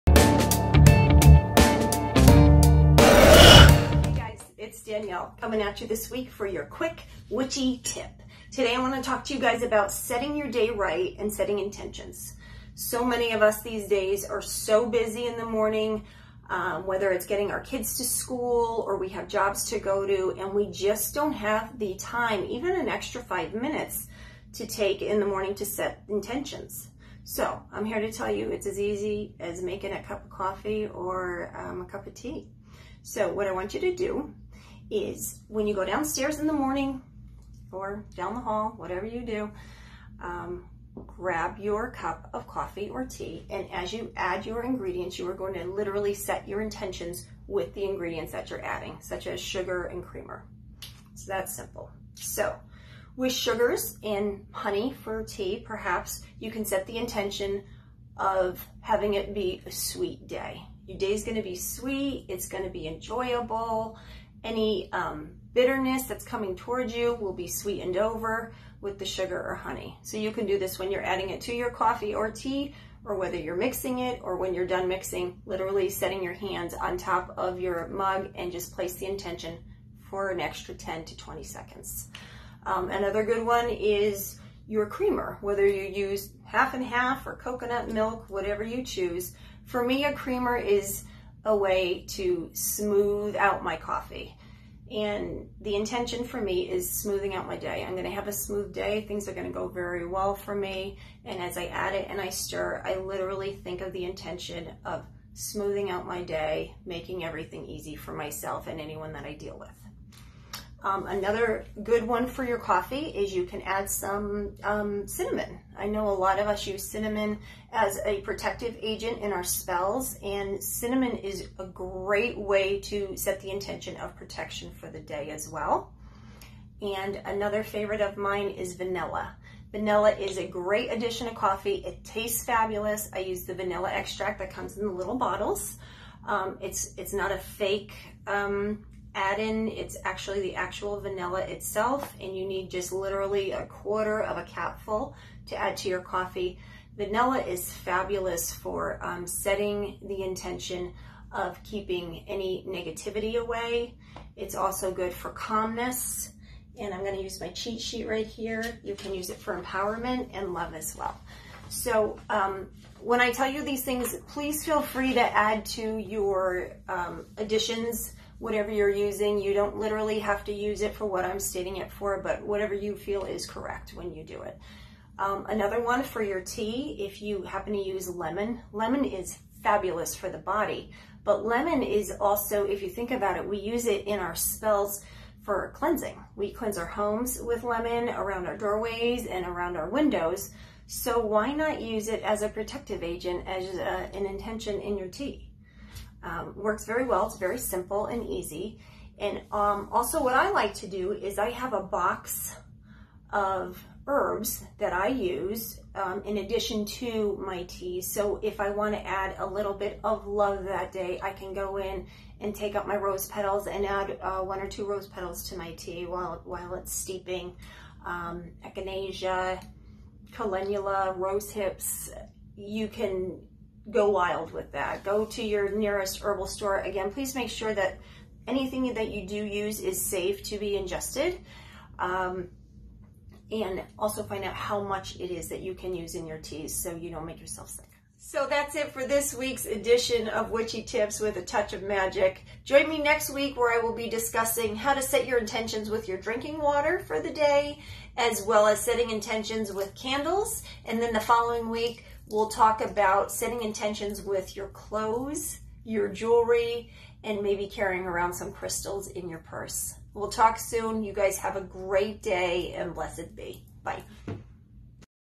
Witch Tips - Easy Ways To Set Daily Intentions - Touch Of Magick
Weekly witchy tips to add a Touch Of Magick to your week w Danielle Nichols

This weeks witchy tips on some easy ways to set intentions in the morning.

Touch Of Magick